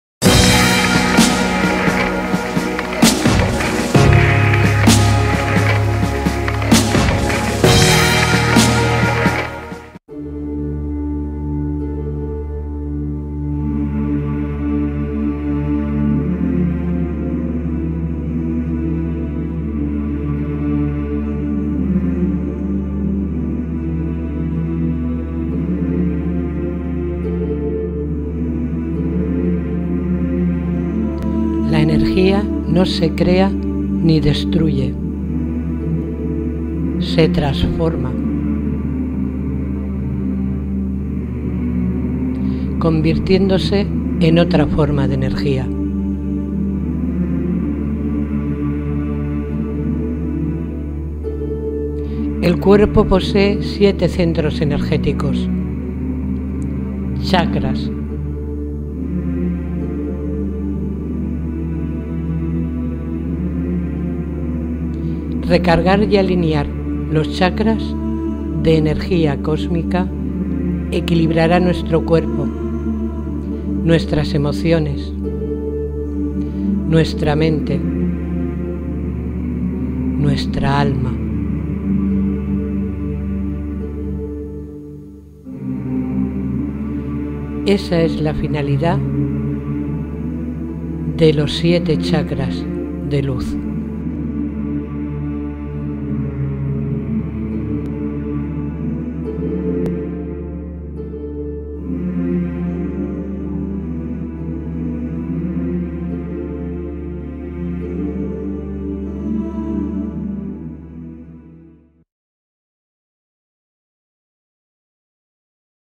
7 Chakras Luz. Meditación con Raquel Llorca
Técnica 1 del método Meditación 7+7, creado por Raquel Llorca. Con esta técnica logramos equilibrar y recargar de energía nuestros centros vitales y cuerpos físico, emocional, mental y espiritual.
raquelllorca.es